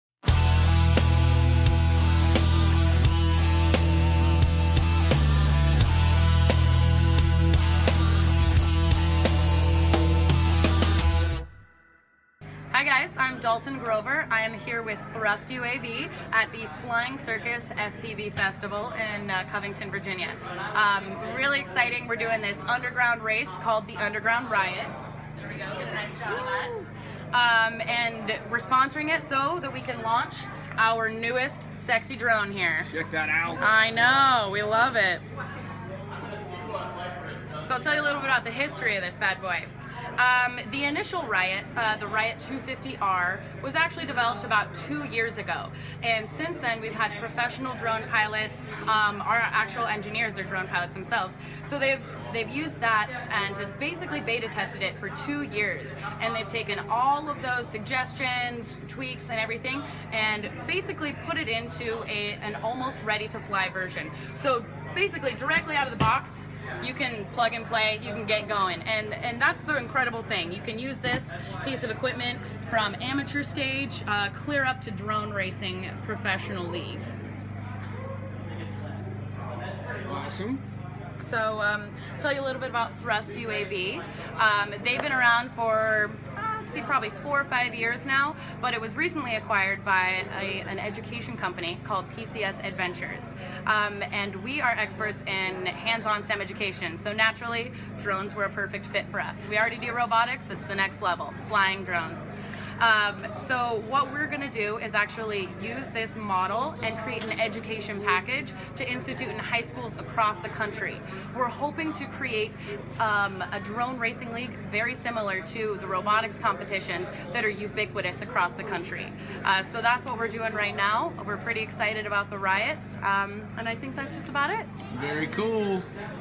Flying Circus ThrustUAV Riot 250 Pro
Dahlton tells us all about the new Riot 250 Pro from Thrust UAV as well some great educational info coming to high schools across america soon.